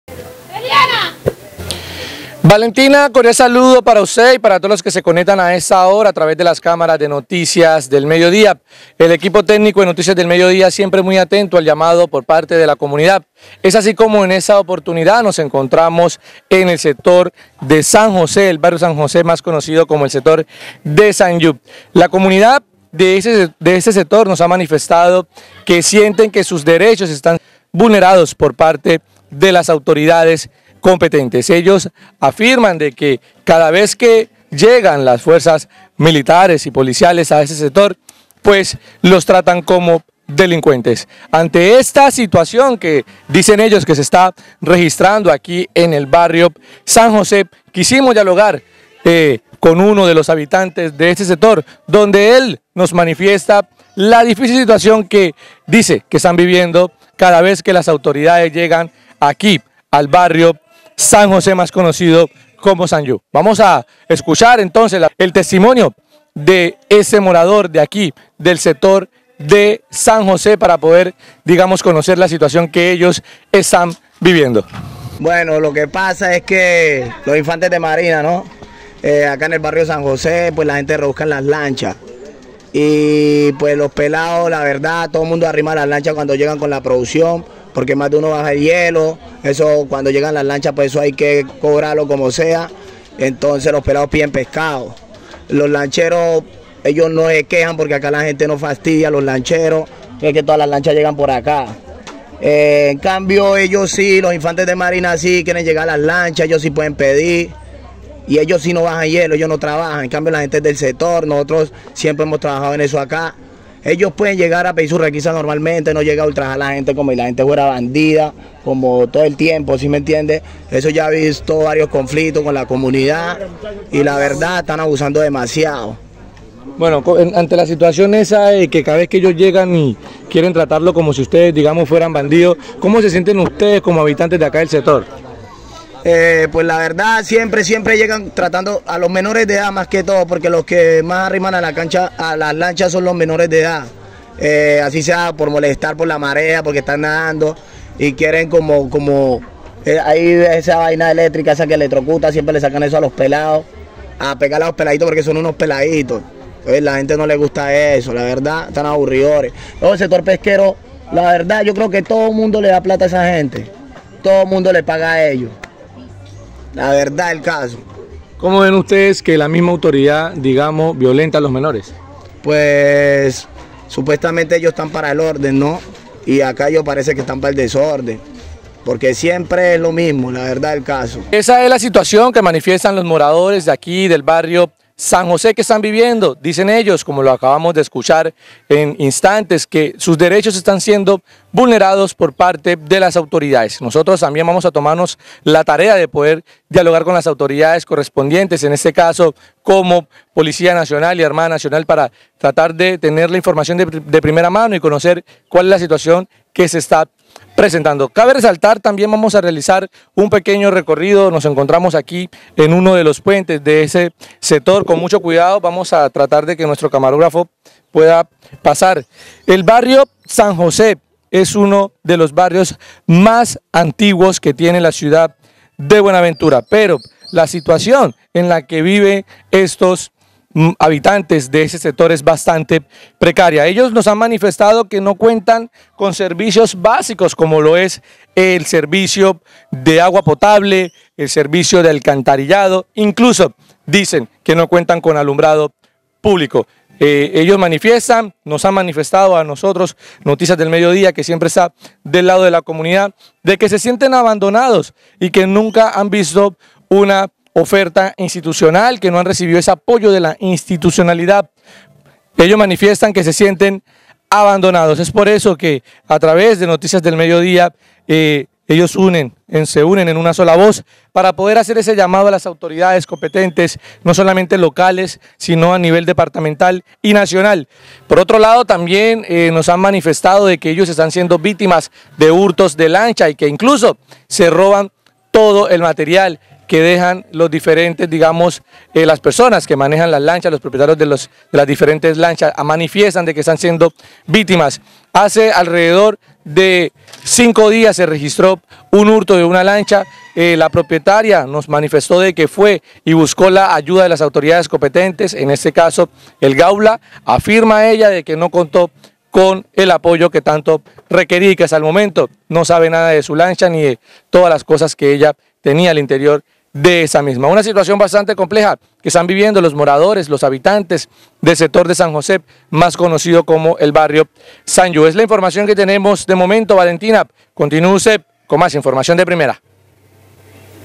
DIR BARRIO SAN JOSÉ
Habitantes de San José denuncian maltrato por parte de las Autoridades.